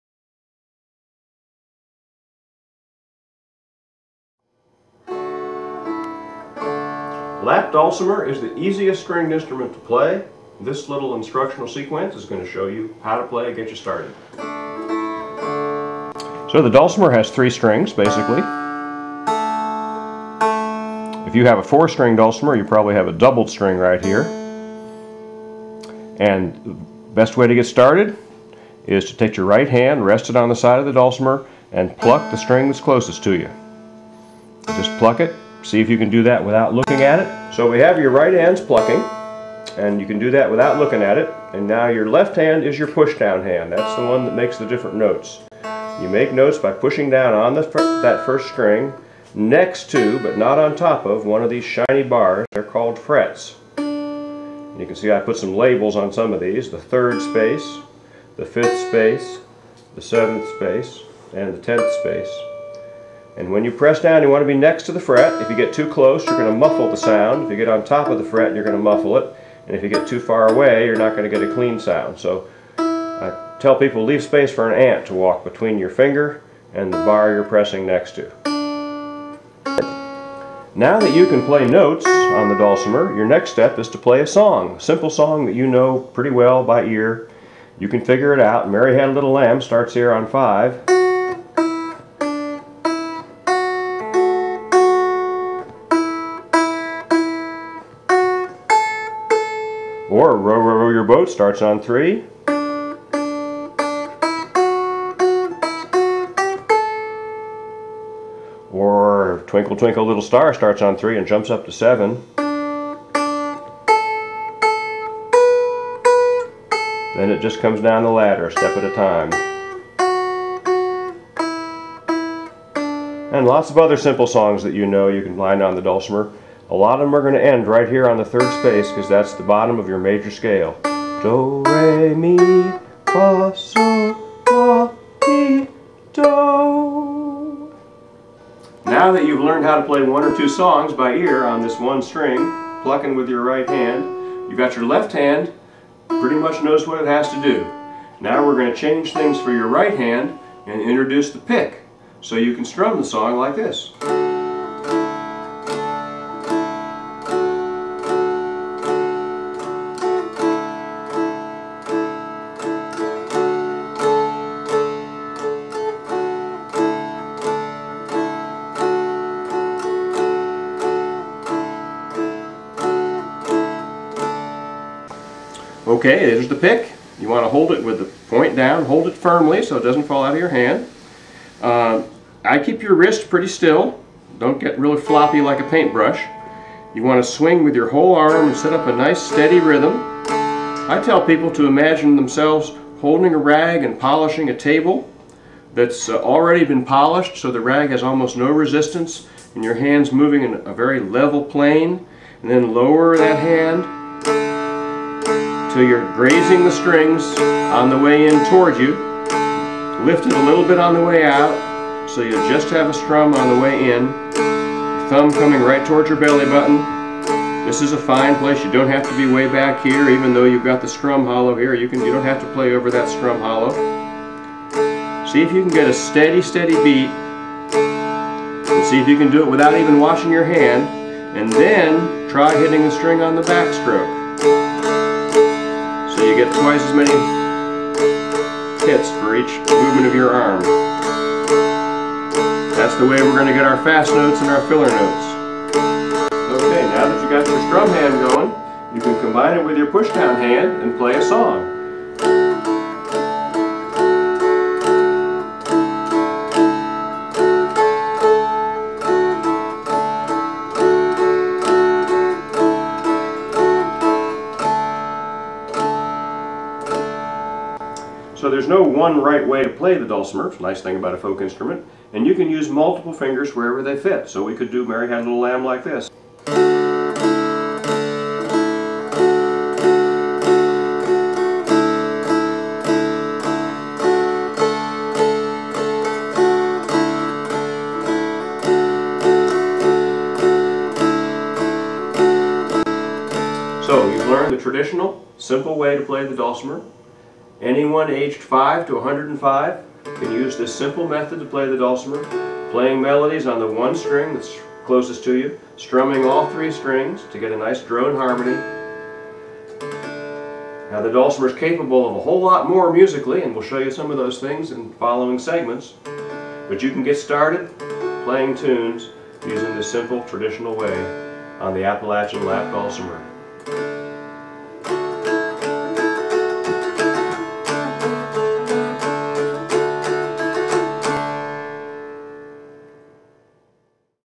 How To Play The Dulcimer
The Appalachian lap dulcimer is the easiest stringed instrument to learn! Play a tune in just five minutes, even if you've never played an instrument before.   David is playing on a Backyard Music Simplicity Dulcimer.